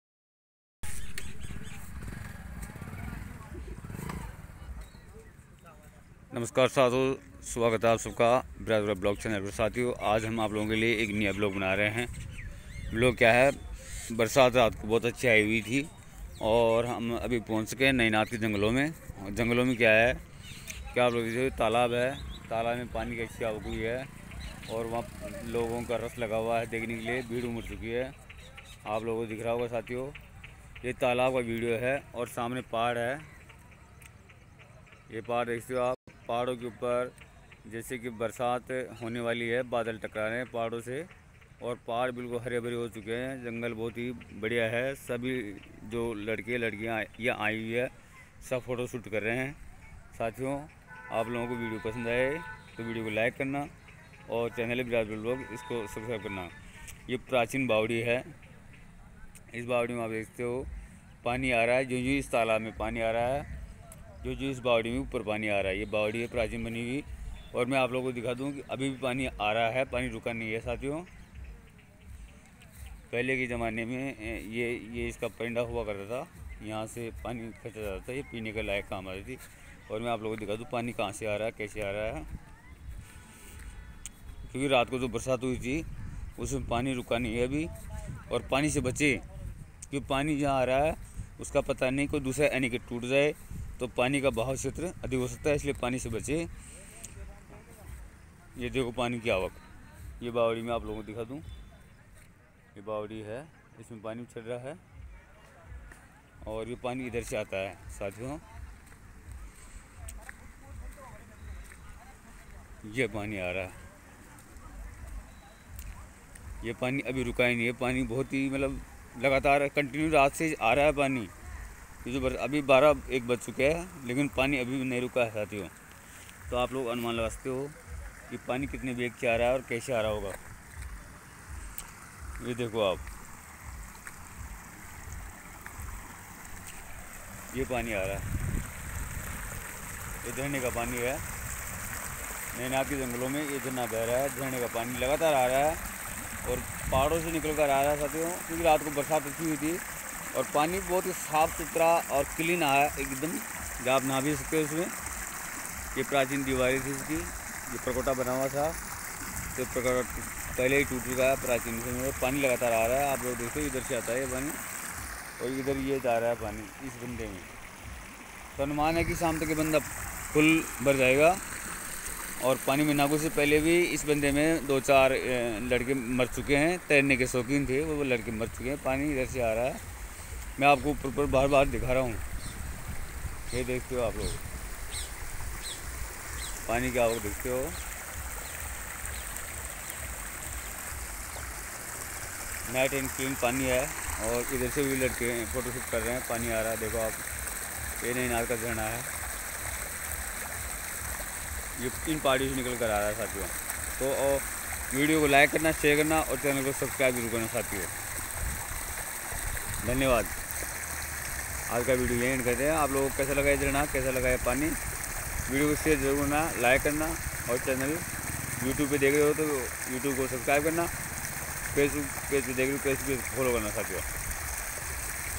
BARSAT KA ZABARDAST VIRAL VLOG VIDEO ll BIRAJPURA VILOGS
नमस्कार साथियों आप सबका स्वागत है मेरे इस ब्लॉक वीडियो में साथियों इस वीडियो के माध्यम से हम आपको दिखाने जा रहे हैं की बरसात की जो पानी होता है उसके बिग का कोई अनुमान नहीं लग सकता सावन के महीने में नए के नाथ महादेव तहसील बस्सी जिला जयपुर पहाड़ी के बीचों-बीच एक छोटा तालाब है उसमें जाने के माध्यम से पानी आ रहा है पहाड़ियों से इसका खूब सुंदर नजारा आप लोगों के सामने है आप लोग पूरा वीडियो जरुर देखना और वीडियो अच्छा लगे तो वीडियो को लाइक करना और चैनल को सब्सक्राइब जरूर करना साथियों धन्यवाद

barsatkavirslvilogvideo#birajpuravilogs